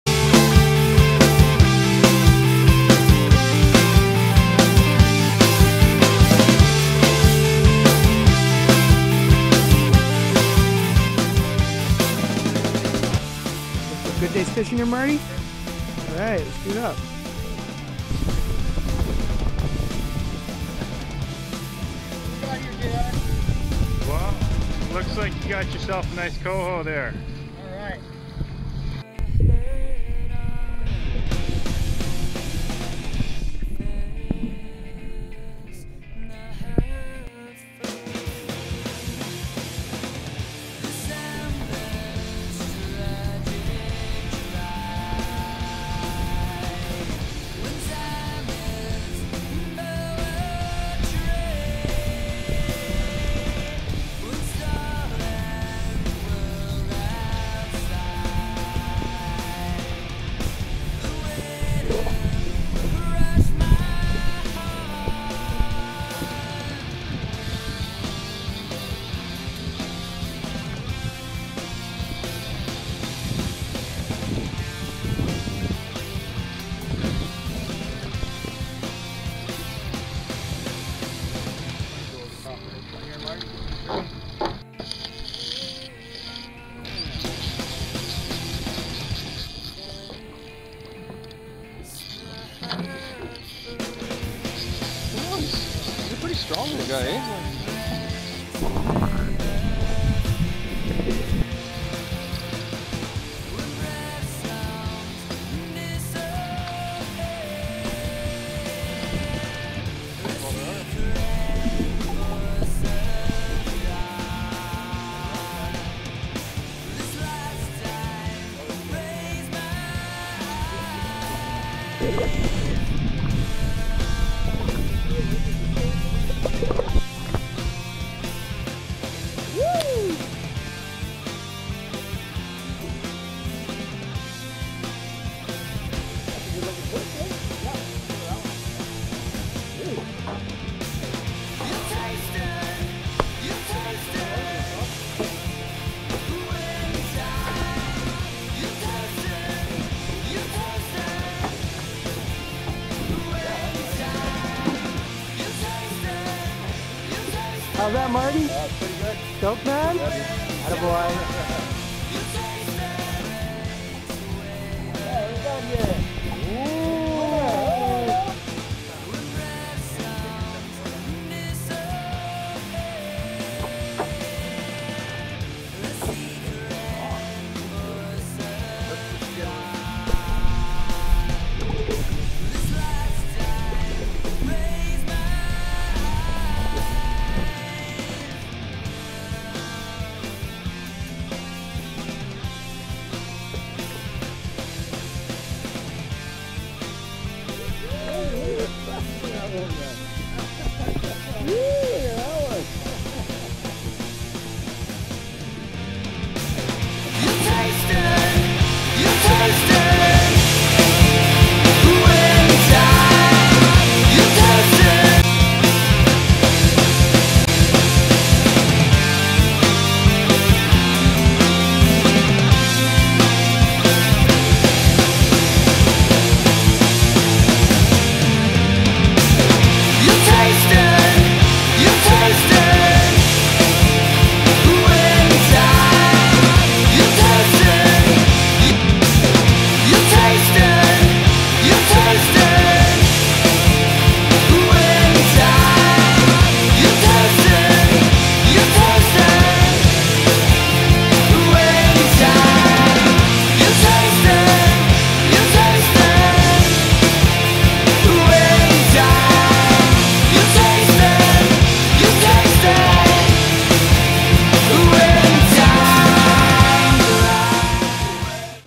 Vancouver Island Coho Fishing Oct 2010
October coho fishing out of Port Renfrew, on the south-west coast of Vancouver Island, with Jolly Rogers Fishing Adventures.  Primetime for salmon fishing here is summer, for chinook salmon, but some of the best fishing action of the year is in Fall, when the big Northern coho come through our local waters.  Here's a fun day chasing coho salmon on a sunny October day.